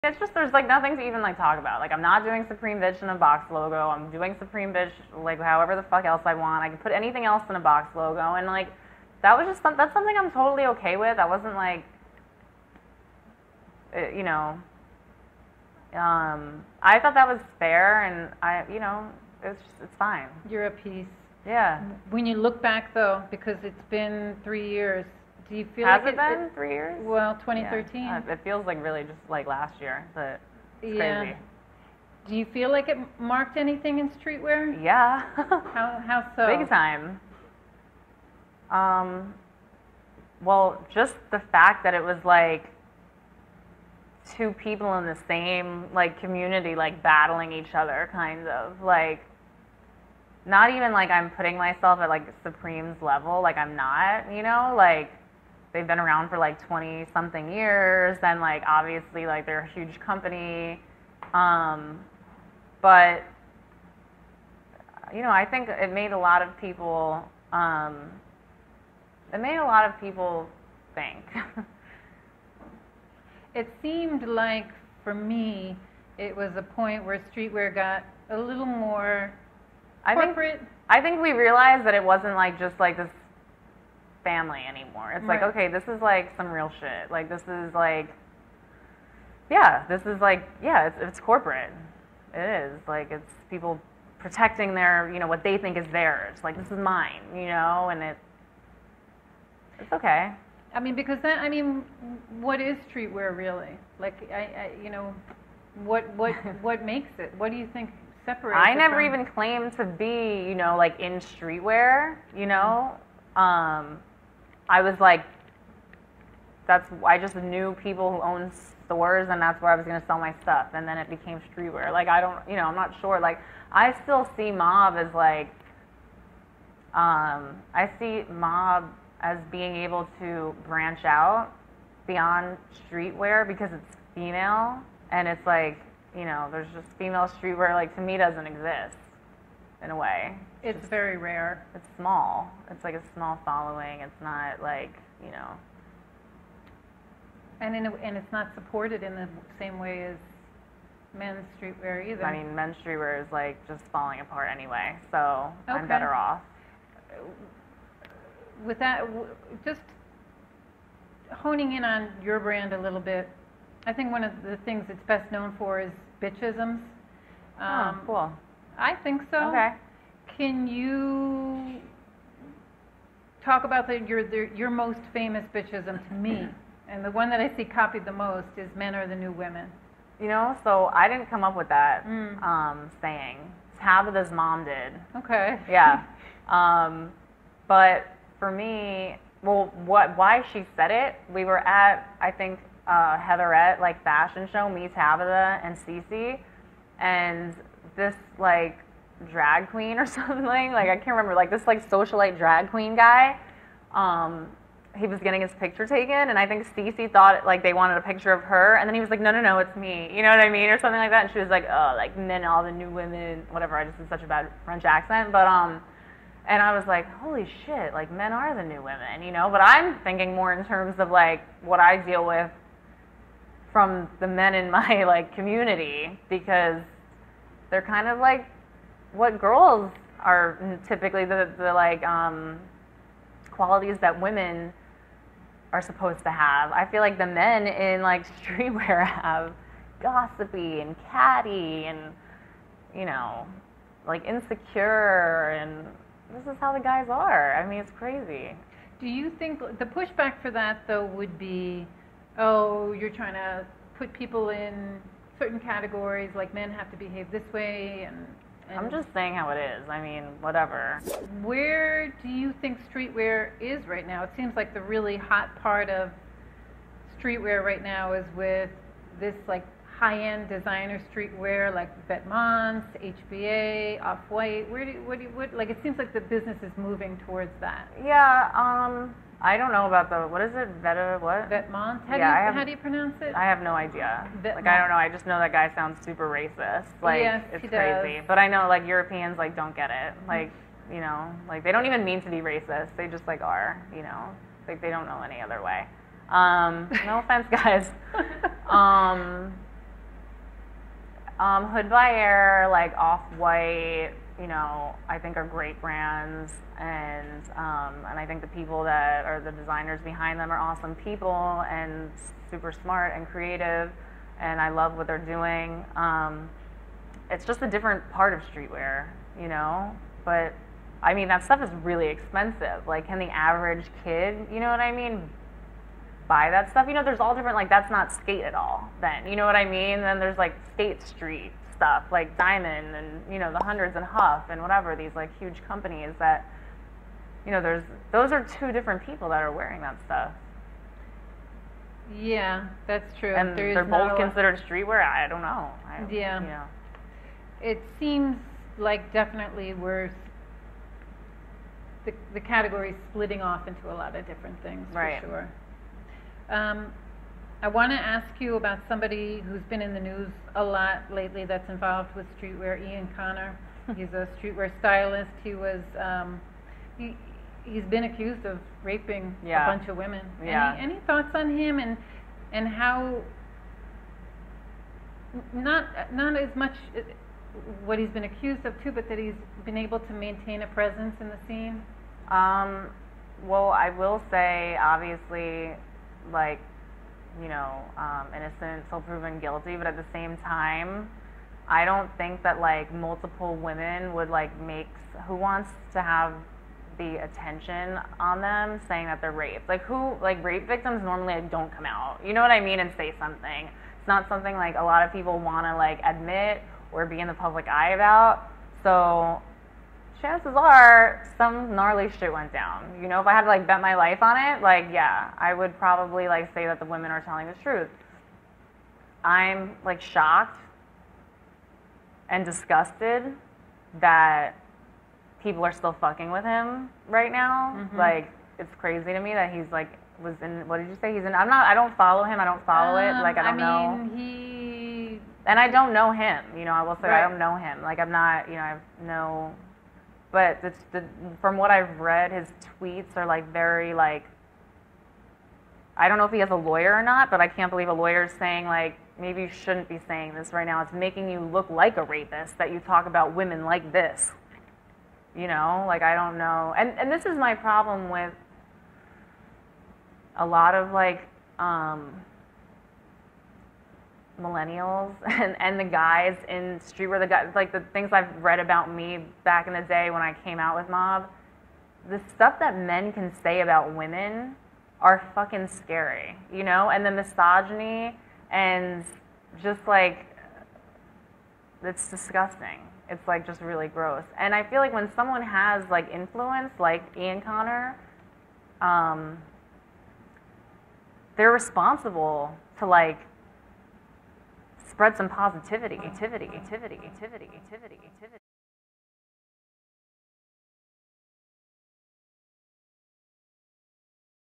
Leah McSweeney: 'I'm Disgusted People Are Still Working With Ian Connor'
In part two of The Snobette's interview with Leah McSweeney, the Married to the Mob founder discusses how being sued by Supreme impacted her brand and streetwear. She also explains the origin of her most famous Bitchism: "Men are the new women." and then segues into the origins of high-end streetwear, questioning whether brands like Off White, HBA and Vetements are authentic streetwear brands. Finally, she checks streetwear stylist Ian Connor, expressing surprise he's been able to continue working with well known labels.

Note: Comments and feedback are very much appreciated, but we do remove any one-dimensional insults or attacks.

Video Production: Mauricio Jorquera